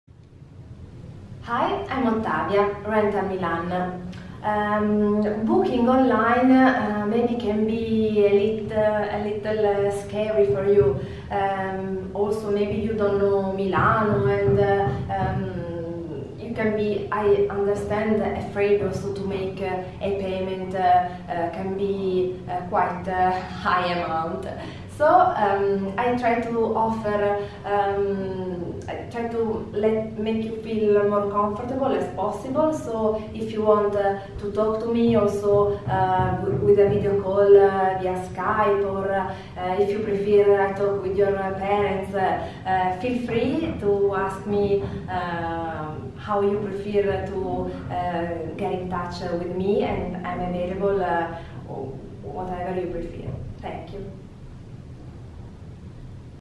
Before booking online we can make video call
Booking online can be little scary for you, also you do not know Milano and I can understand you can be afraid also to make a payment
I can try to make you feel as comfortable as possible. I could talk with you also video call with Skype / Whatsapp or FaceTime whatever what you prefer and I am available also to talk with your parents if you need.

All the details and the 360° Virtual Tour